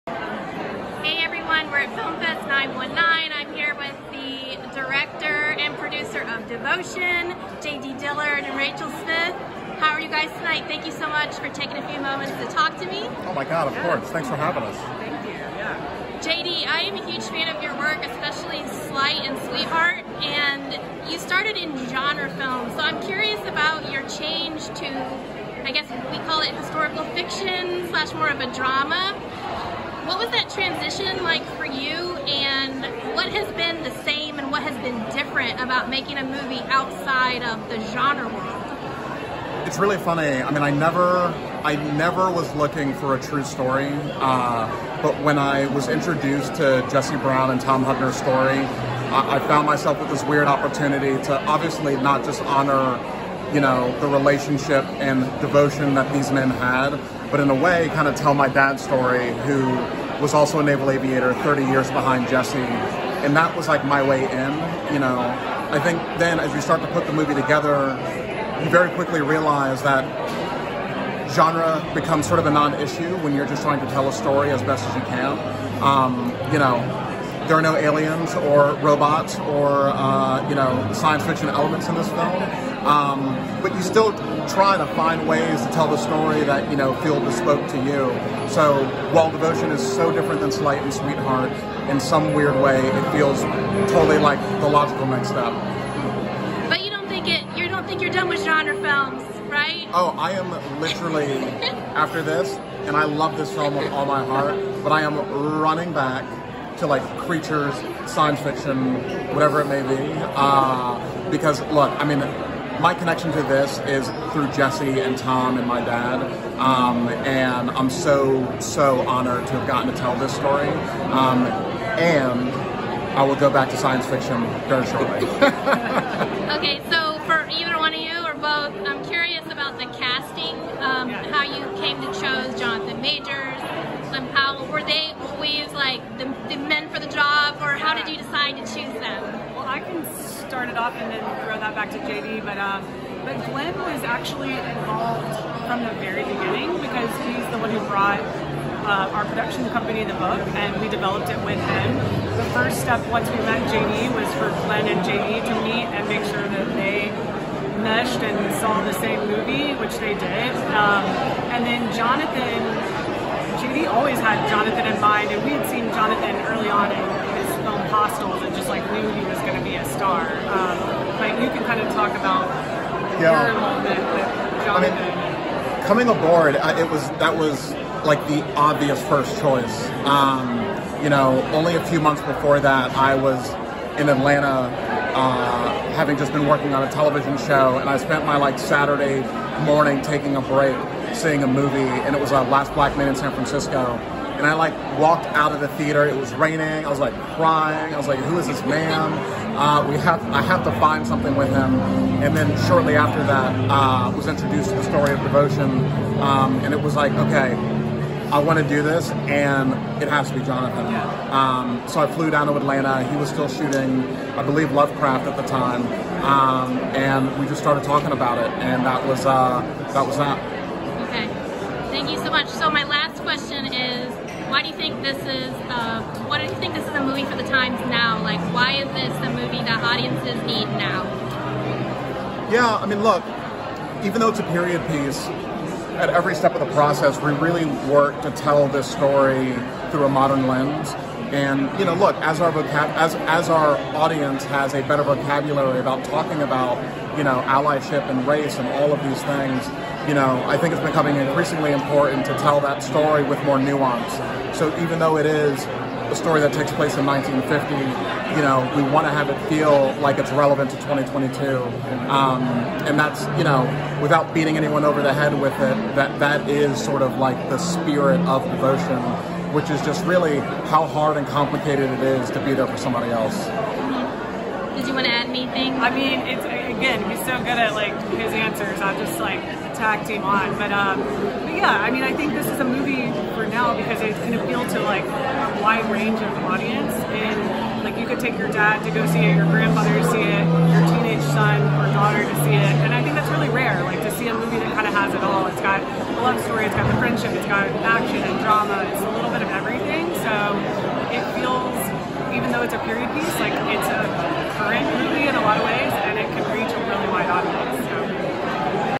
A Conversation with JD Dillard and Rachel Smith, director and producer of Devotion [Film Fest 919]
The 2022 Film Fest 919 kicked off the week with a screening of Devotion, directed by J.D. Dillard and starring Jonathan Majors, Glen Powell, and Daisy Brown. Based on a best-selling book with the same title by Adam Makos, with screenplay by Jake Crane and Jonathan Stewart, Devotion is the true story of Jesse Brown (Majors), the first Black aviator, and his relationship with his wingman, Tom Hudner (Powell).

Dillard, along with producer Rachel Smith, attended the screening at the Silverspot Cinema in Chapel Hill, NC. It was the first live interview for 1 of My Stories, and I was instructed to line up at the "step and repeat" in order to speak to Dillard.